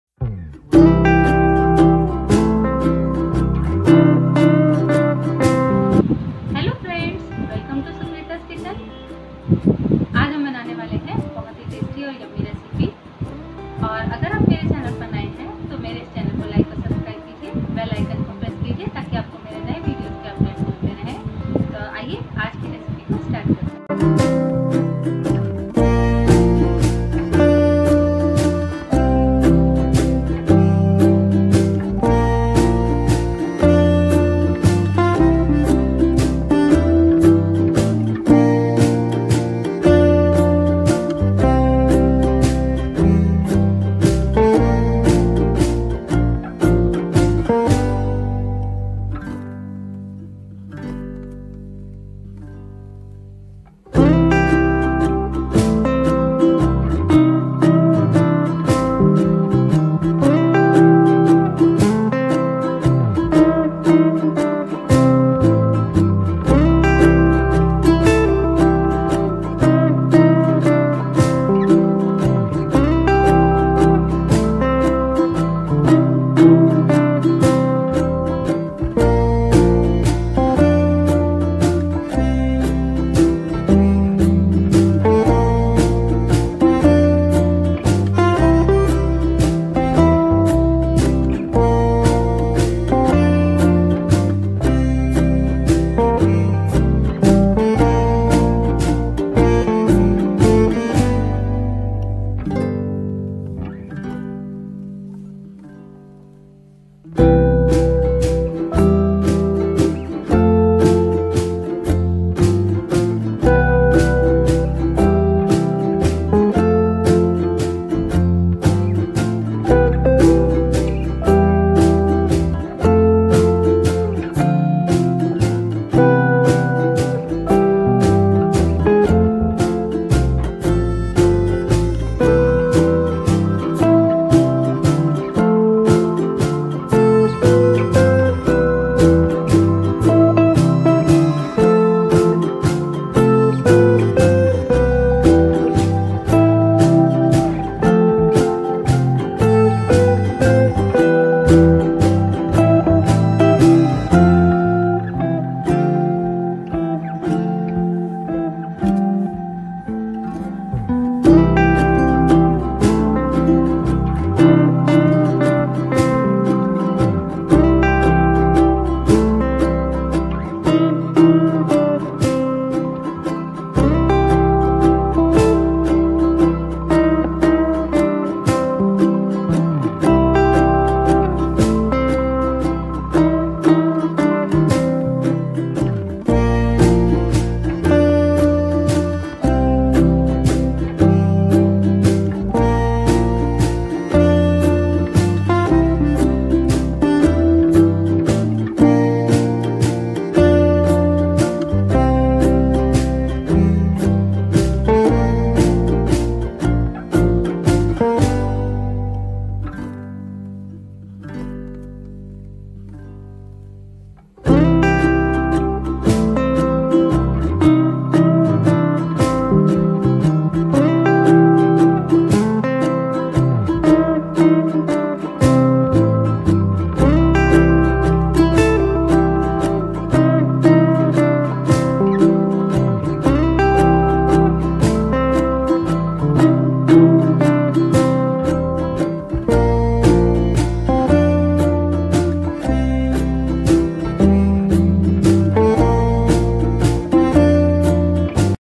Seasame Toast | How to make Seasame Toast | Seasame Toast Recipe | Til ke toast | तिल के टोस्ट
Try this evening time snack with your family. Hope u all love it
तिल के टोस्ट